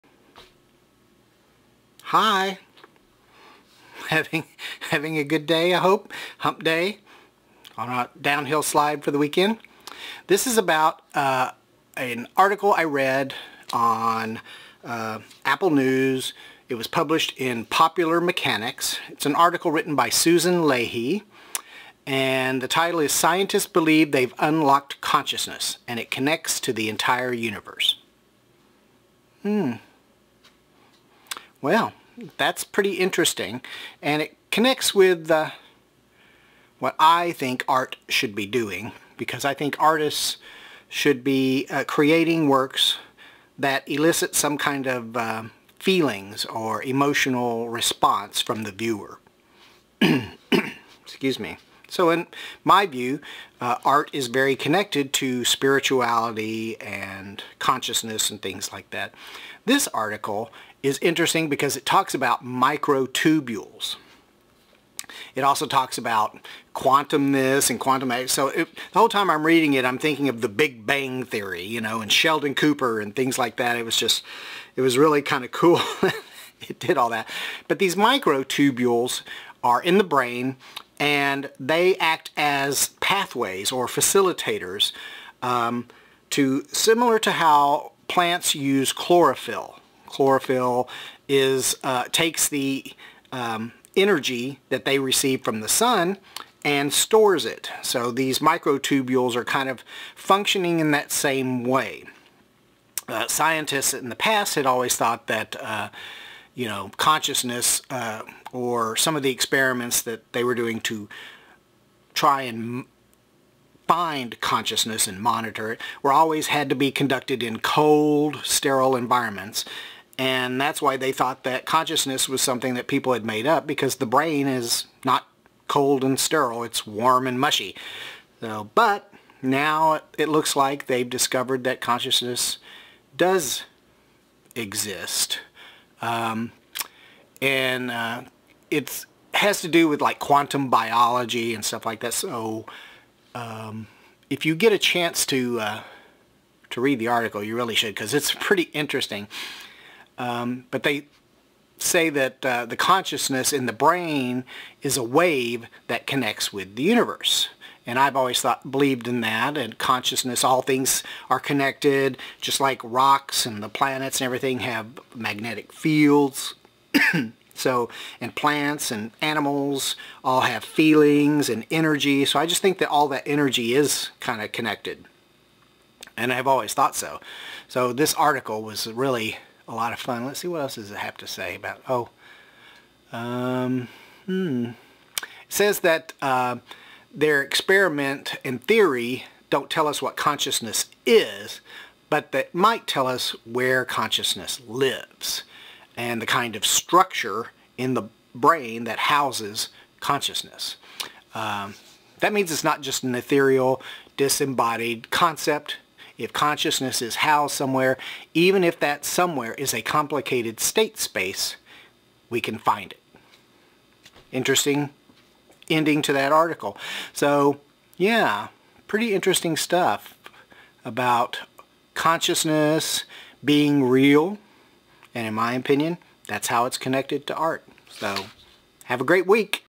ART Spirituality - Consciousness
A great article from Susan Lahey published in Popular Mechanics titled - Scientists Believe They've Unlocked Consciousness - and It Connects to the Entire Universe

Give it a read, I think consciousness is directly related to Art.  Let me know what you think.